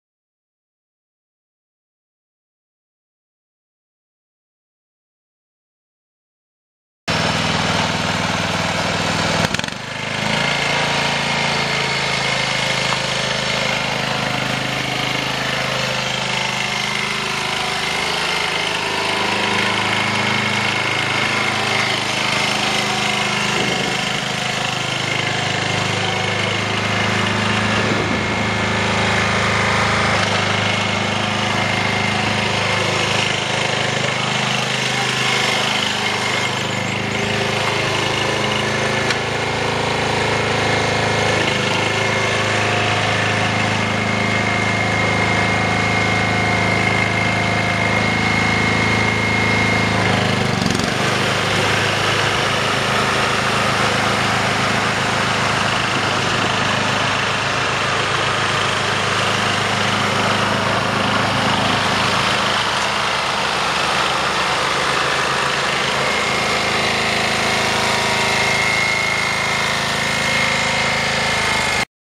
MTD Yard-Man Yardbug lawn mower for sale | sold at auction May 29, 2013
This asset was for sale and sold for a contract price of $110 in an absolute, no-reserve Internet auction by Purple Wave on May 29, 2013

MTD Yard-Man Yardbug lawn mower
30" deck
Briggs & Stratton single cylinder 8.5 HP gas engine
Model 198707
Under seat bagger
11 x 4.00-4NHS front tires
16 x 6.50-8NHS rear tires
Front tires show wear
Small hole in steering column
Seat cracked
Weak battery

It was located in Columbus, NE, and we sold it to the highest bidder for $110.